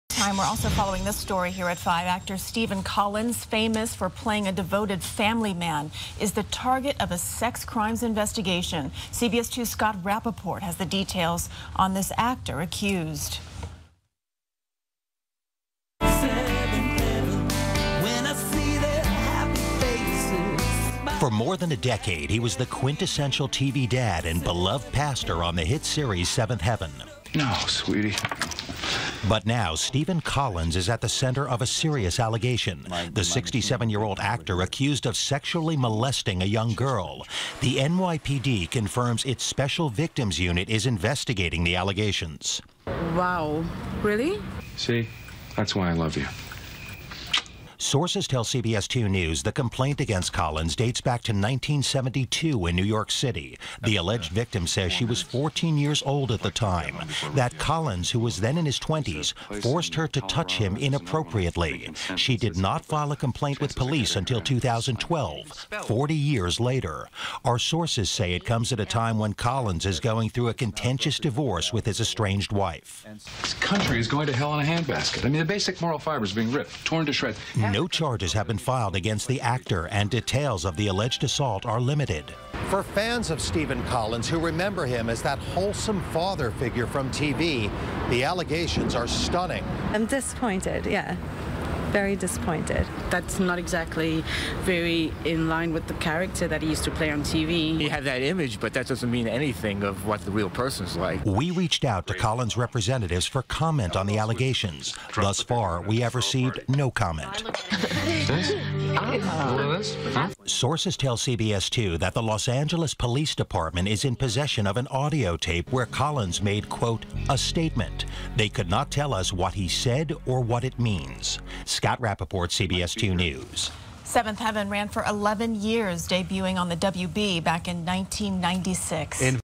Actor Stephen Collins Under Investigation For Possible Sex Crimes
CBS 2's Scott Rapoport has more on the accusations against the 7th Heaven actor.